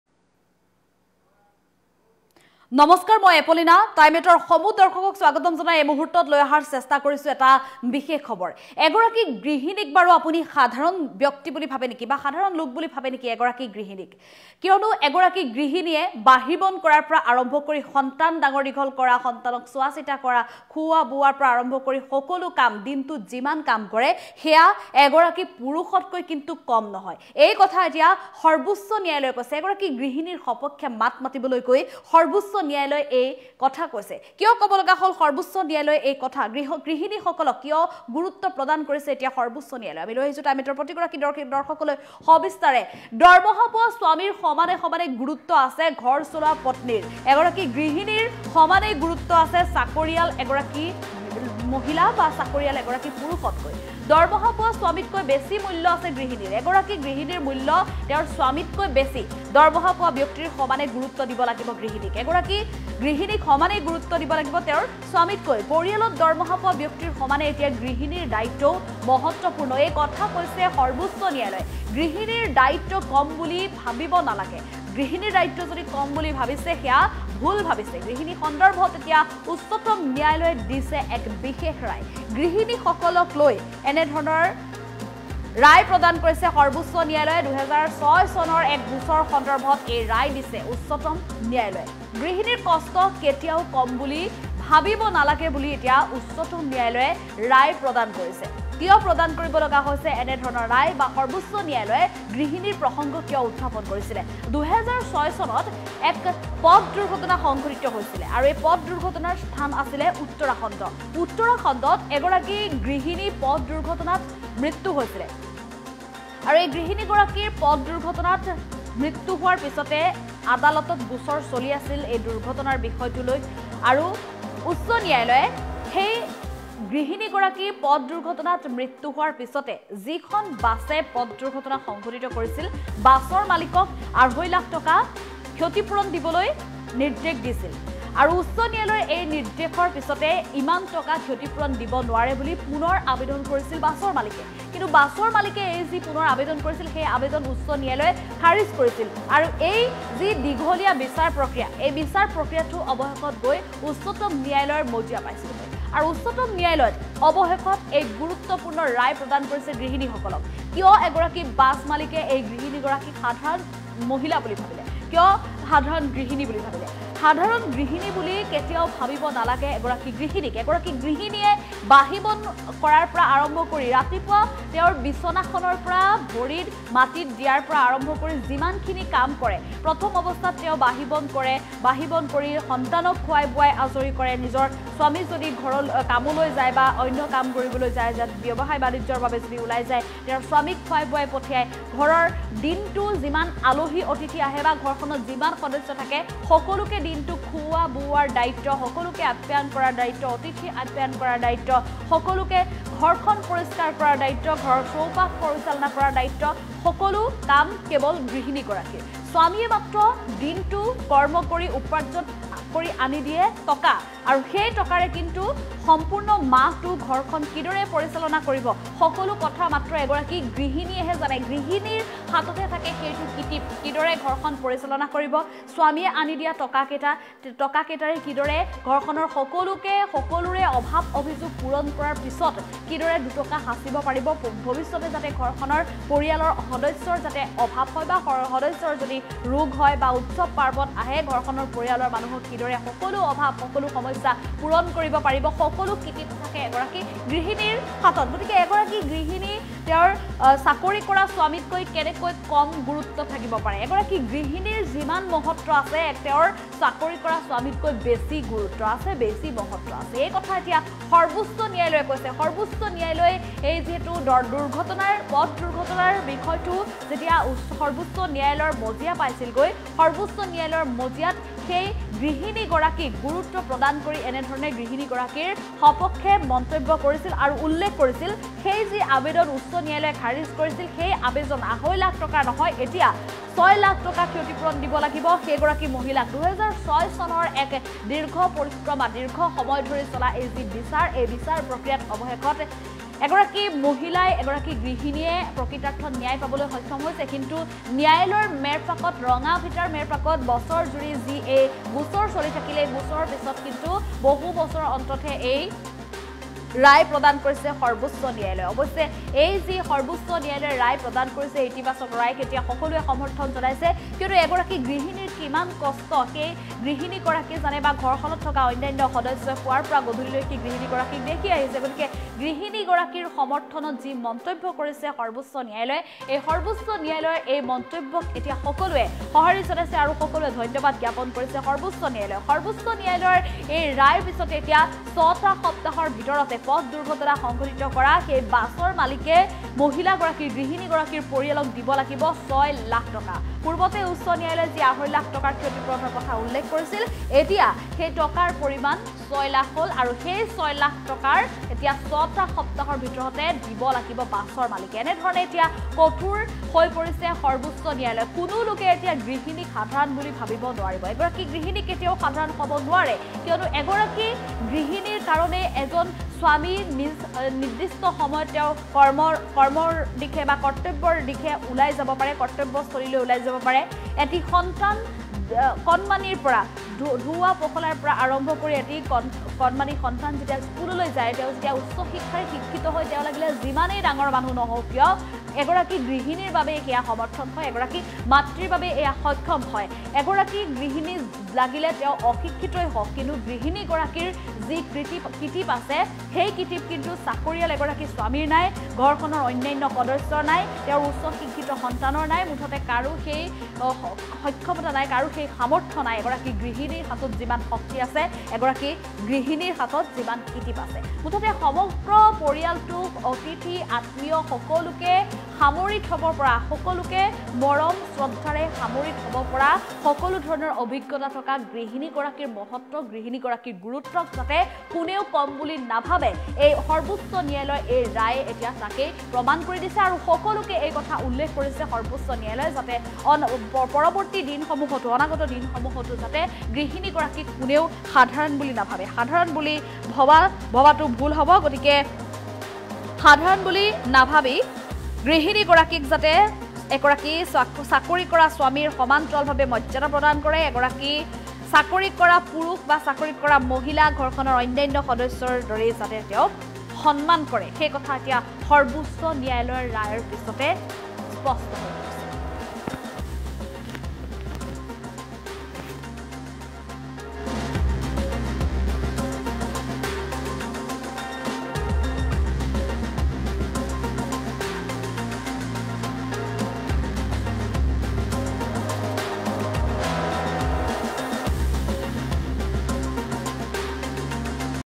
চাকৰি কৰাসকলৰ সমানেই মূল্যবান গৃহিণীসকল,
চাকৰি কৰাসকলৰ সমানেই মূল্যবান গৃহিণীসকল, উচ্চতম ন্যায়ালয়ৰ ৰায়দান, 'দৰমহা পোৱা ব্যক্তিৰ সমানে গুৰুত্ব দিব লাগিব গৃহিণীক'